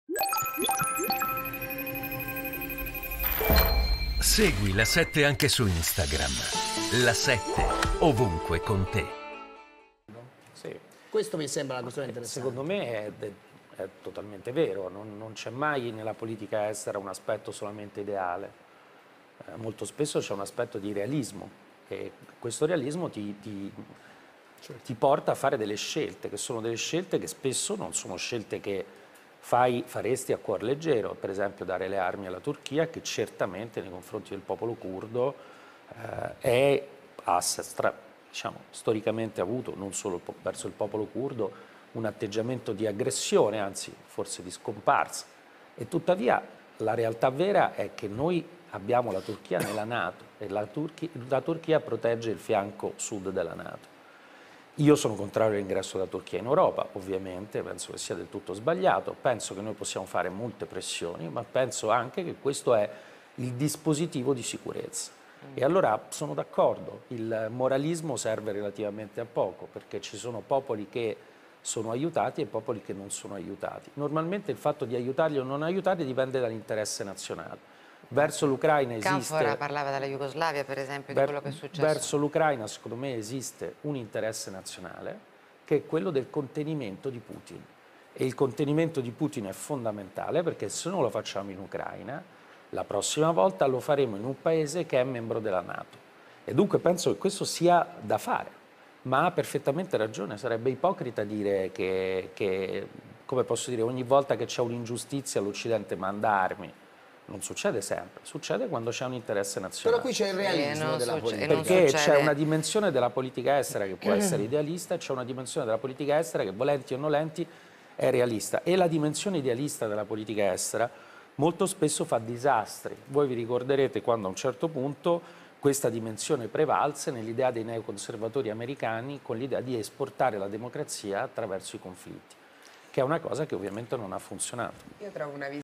Ucraina, Calenda: "Se non conteniamo Putin in Ucraina la prossima volta lo faremo in un Paese ...
Carlo Calenda: "Il contenimento di Putin è fondamentale, se non lo facciamo in Ucraina la prossima volta lo faremo in un Paese membro della Nato"